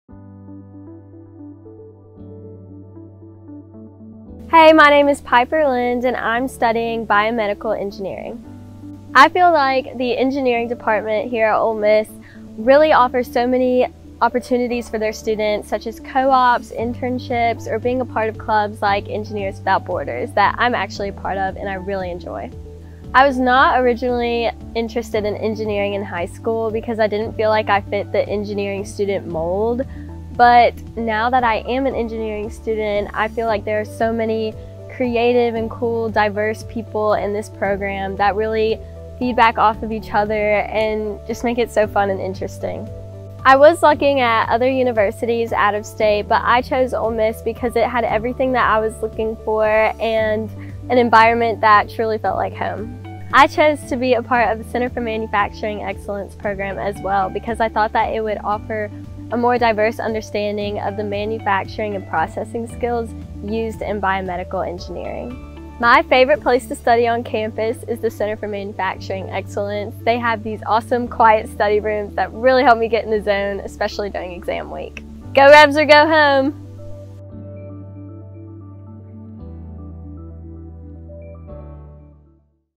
The engineering experience
Biomedical engineering student Piper talks about the creativity and diversity of her fellow students, all the student orgs Ole Miss Engineering offers and other academic pursuits.